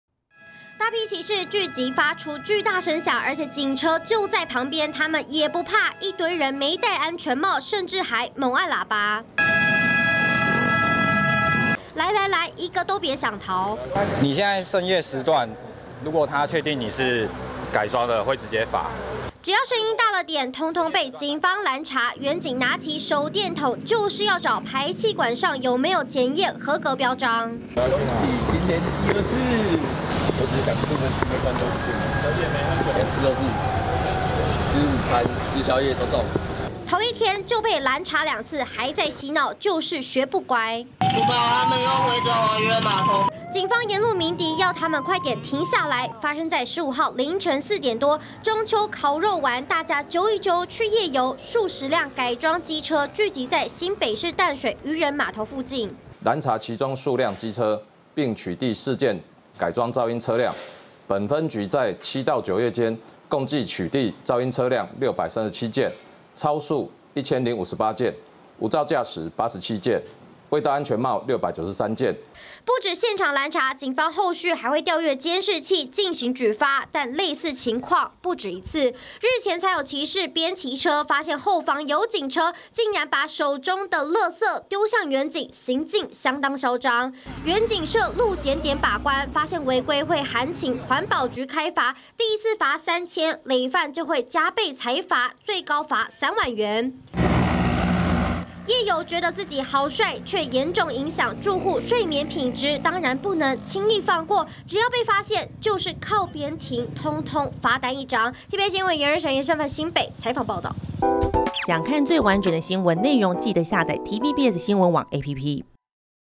按喇叭挑釁! 飆車族「漁人碼頭」 警網攔截｜TVBS新聞
周末凌晨，大批飆車族中秋烤肉完，聚集在新北市淡水漁人碼頭附近夜遊，沿路催油門，看見警車還猛按喇叭挑釁，陸續被警方攔下，不只現場，後續也會調閱監視器進行舉發。